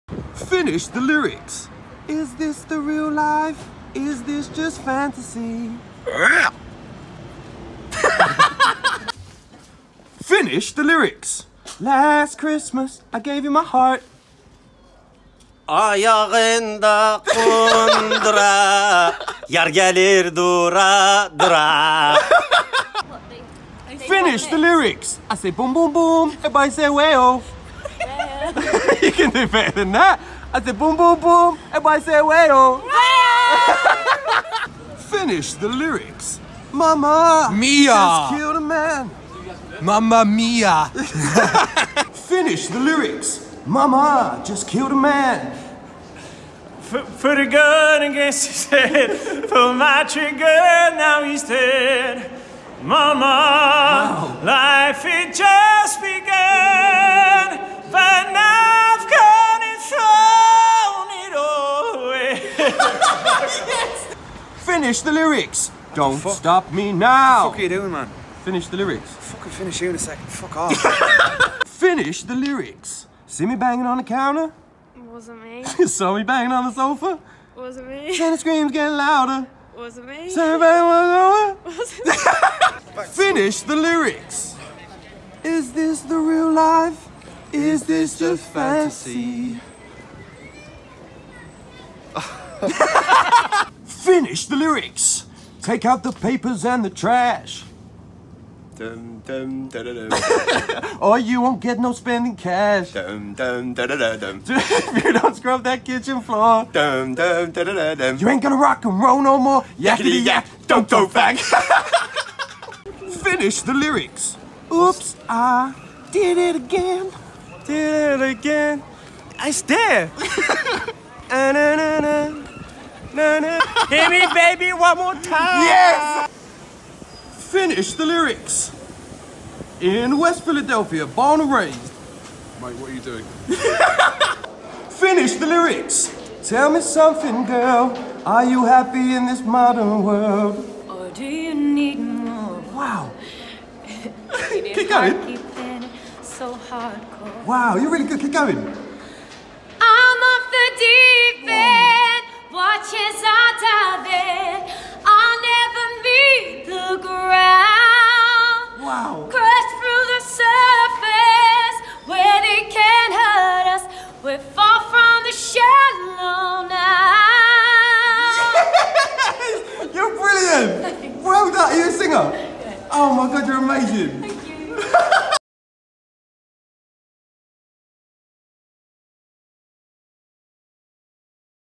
Kevin Freshwater - FINISH THE LYRICS!
I never expected to bump into such talented people while filming Finish The Lyric in London!